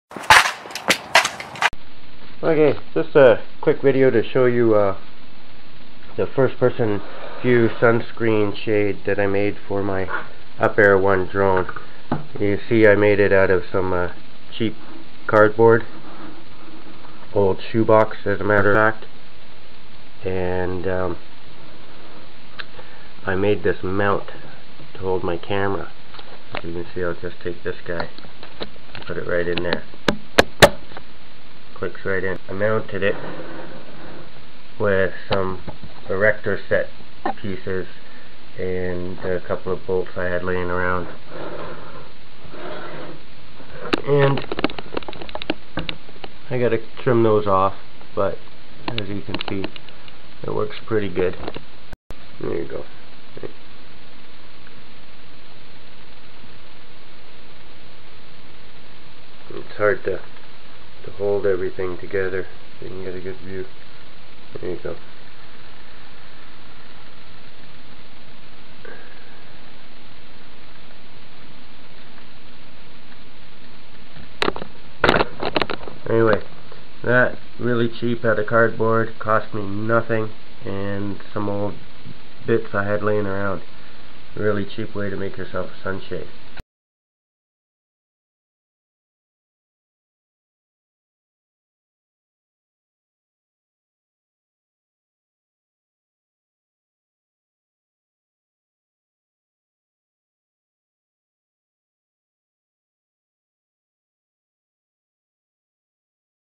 DIY UPAIR ONE Sunshade With camera Mount.
My Creation of a sunshade with a camera mount for free.

Here's a list of topics;
shtf, survival, apocolyps, how to, diy,upair one, drone, kdenlive, linux, ubuntu.